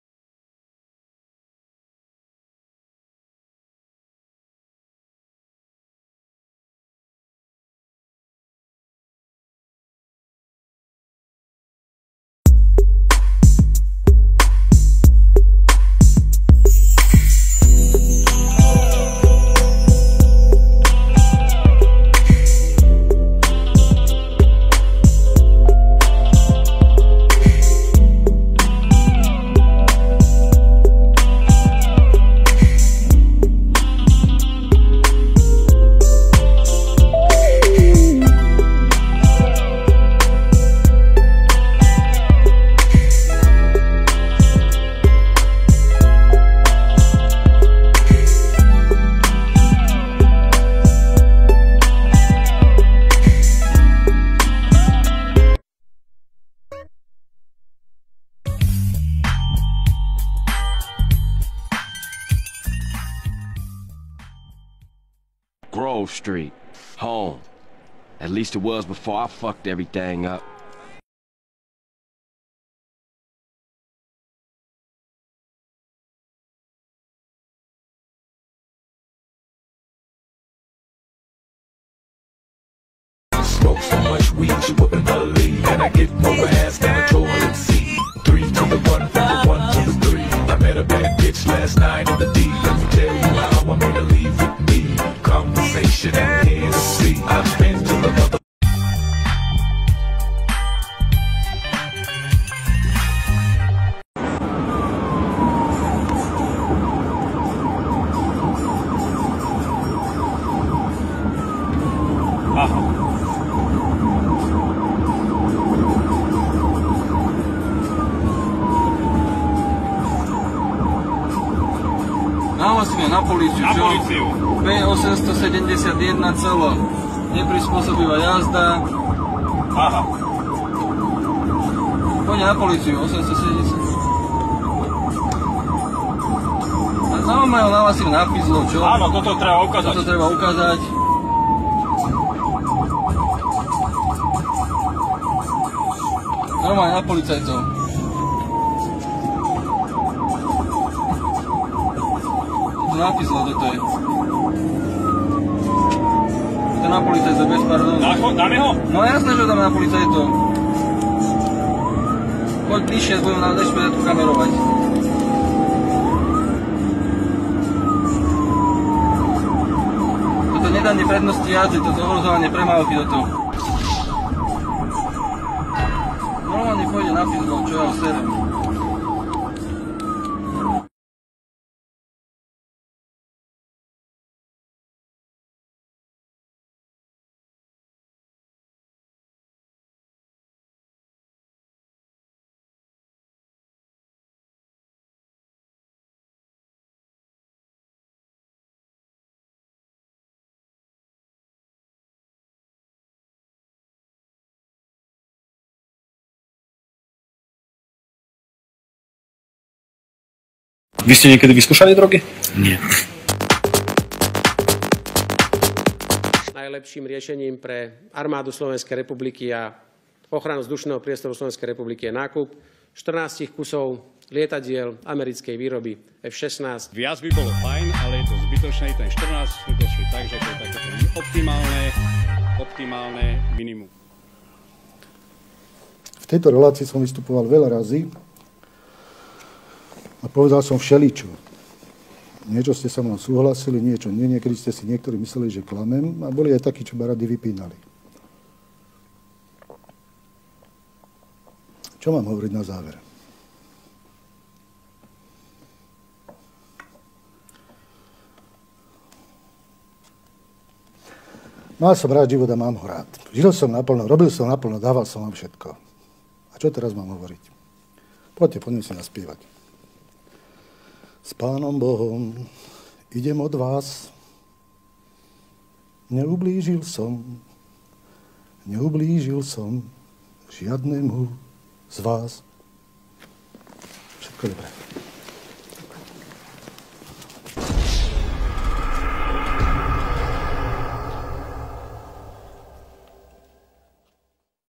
GTA SLOVAKIA
The perfect game done by Slovak politicians and we all want to play this game.